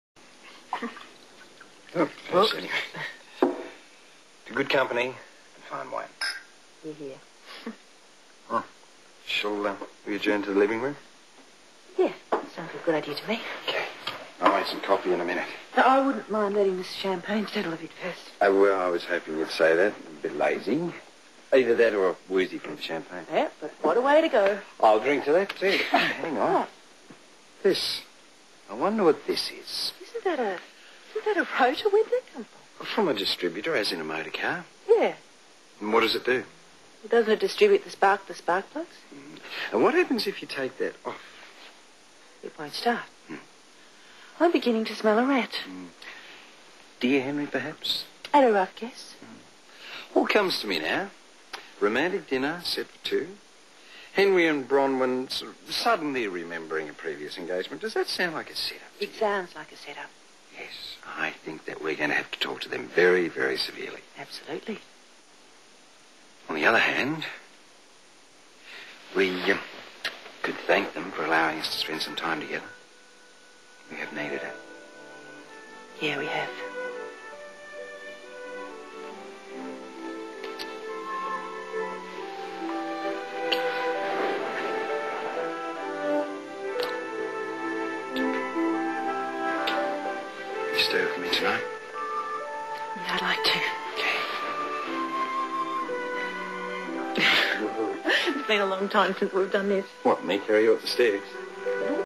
Paul and Gail reconcile
After Henry leaves the pair alone, Paul and Gail recocnile.